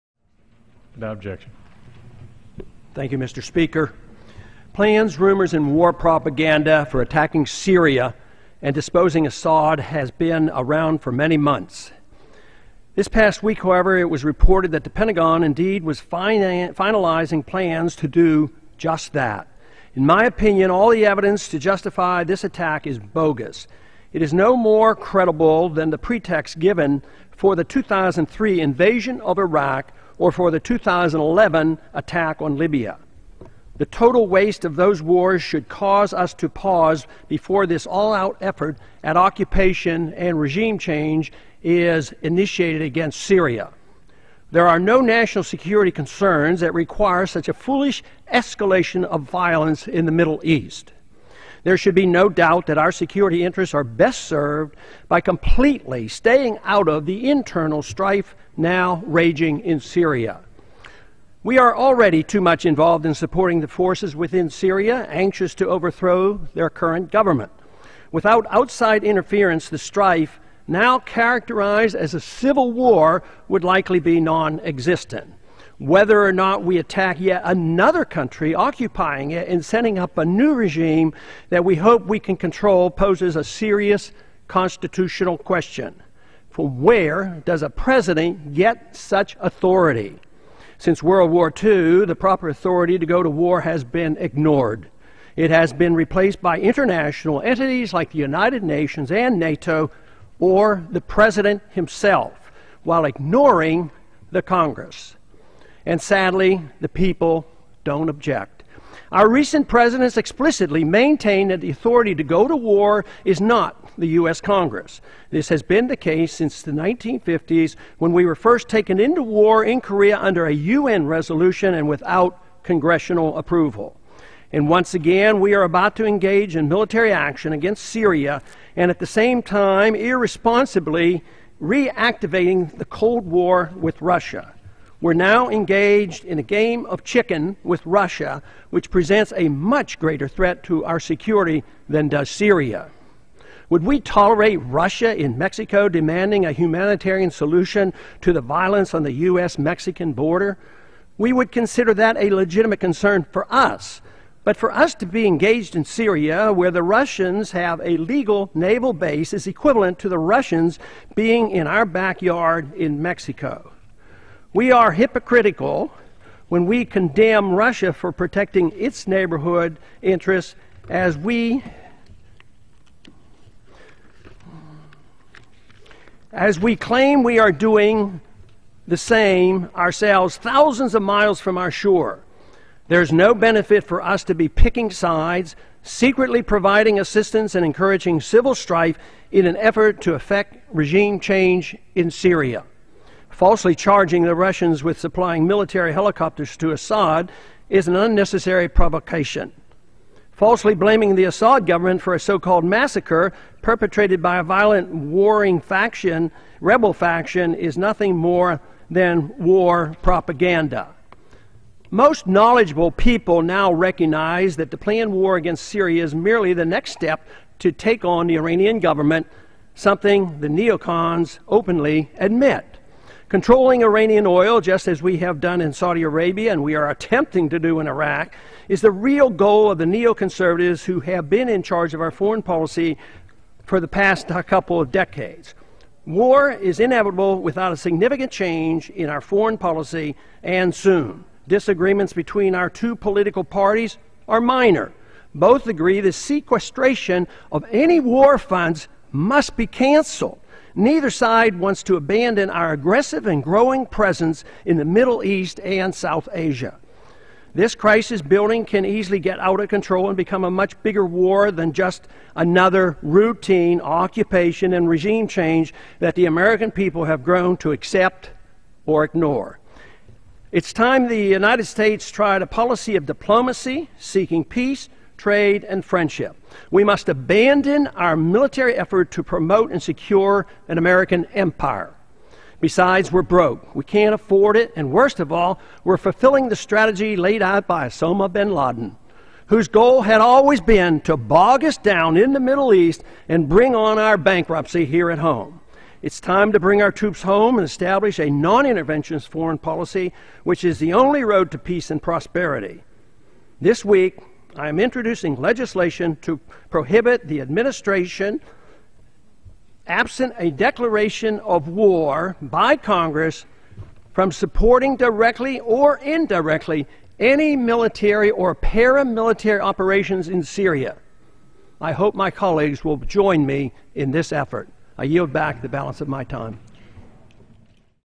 Ron Paul No to War With Syria
Obama and his administration want to send our troops to war in Syria without declaring through Congress. This type of imperialism must stop! We can't afford and people die.